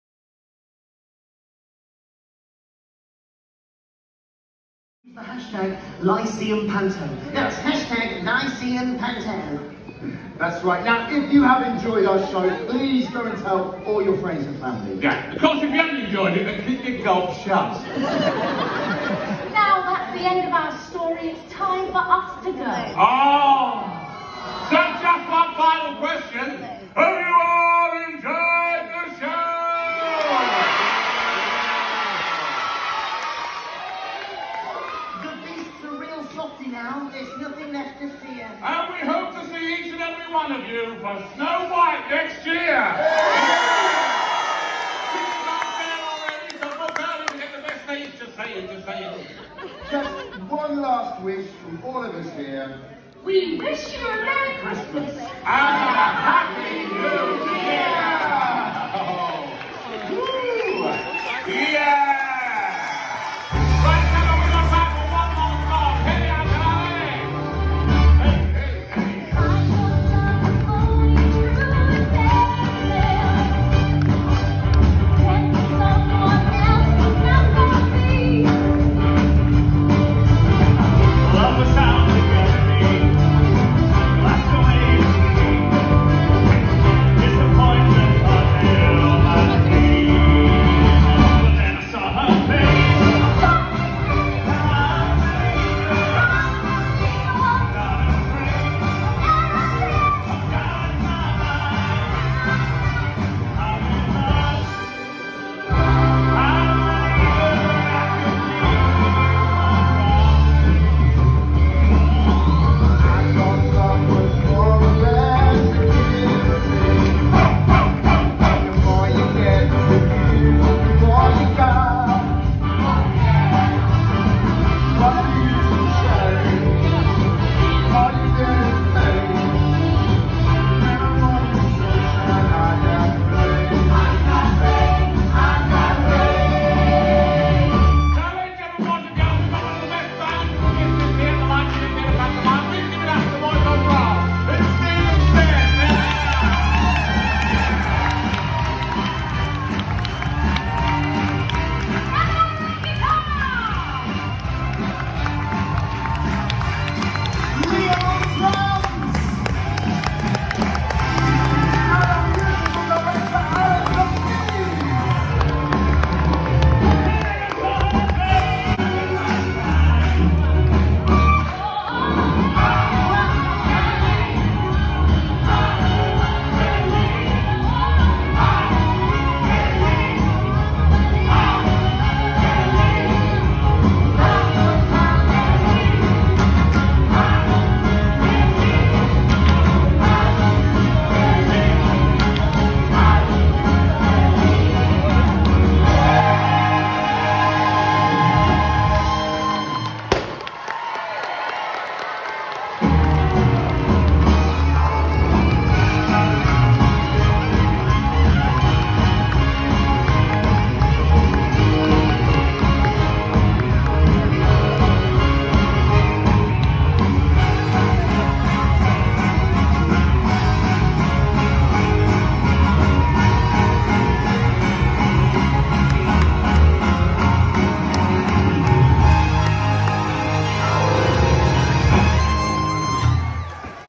Sheffield Lyceum, Beauty and the Beast
Starring: Duncan James as Danton
Damian Williams as Madame Bellie Fillop
Jennie Dale as Cupid
Max Fullam as Phillipe Fillop
Bessy Ewa as Belle
Aidan Banyard as Beast/ Prince

Ensemble: Ronnie Burden, Luke Cartledge, Hollie Duncan, Shona Mairi Masson, Lavinia Mai, Rhys Wyn-Owen and Kate Roddy.